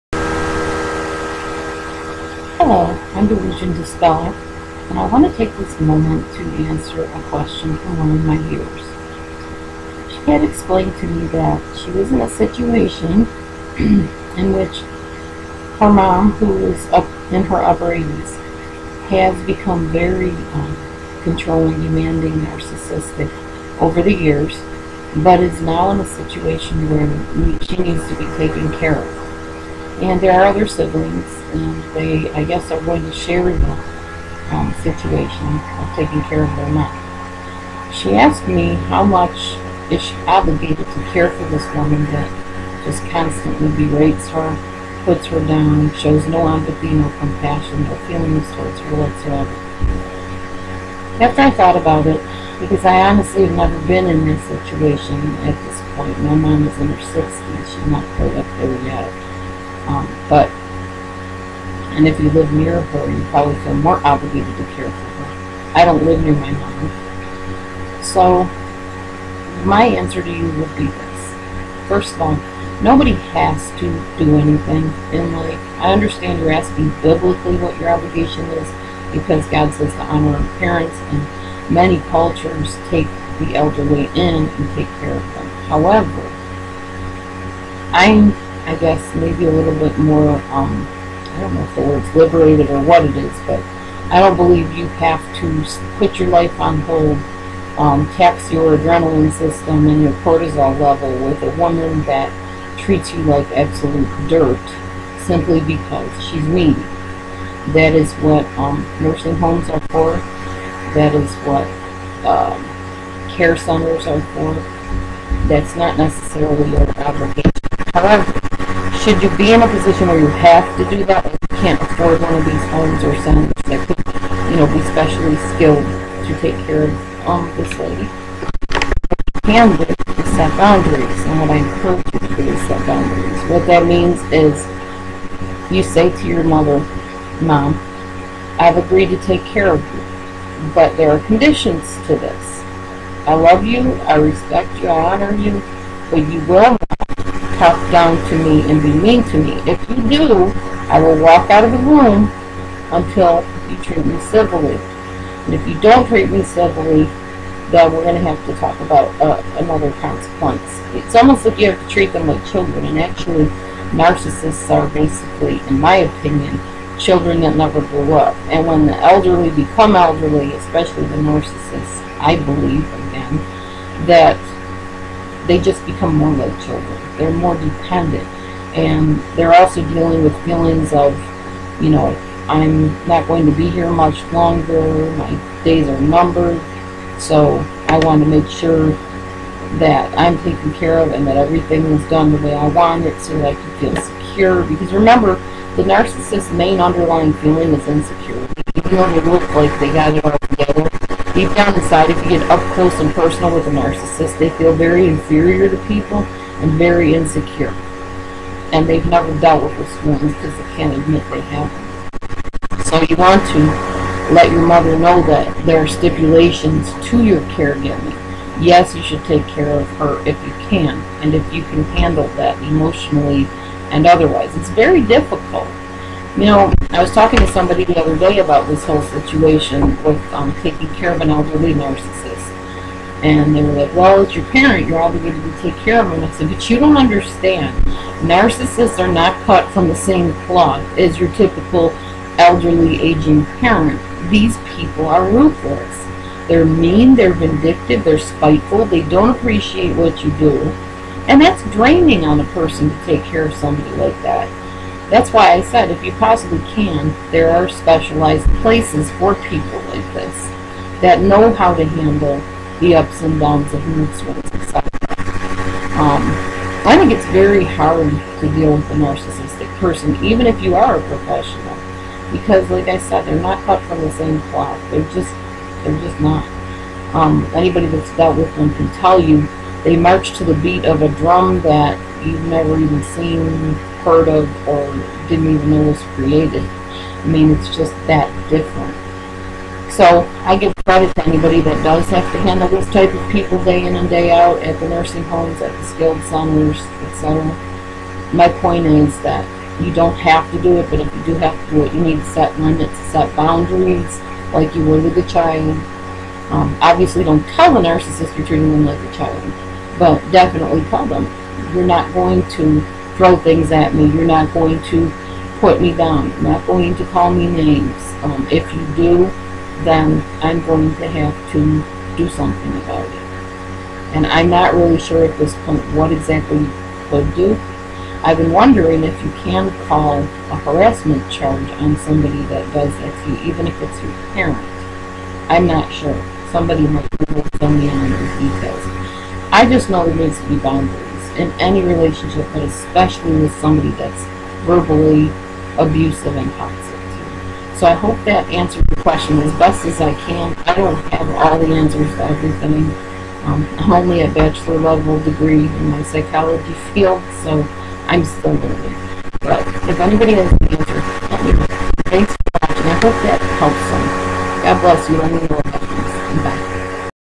HOW TO Handle Your Aging Narcissistic Egg/Sperm Donor
A viewer asked me what her obligation was to her narcissistic [mother]. I did the best I could to answer in this video. All other suggestions to this person are very welcome and appreciated.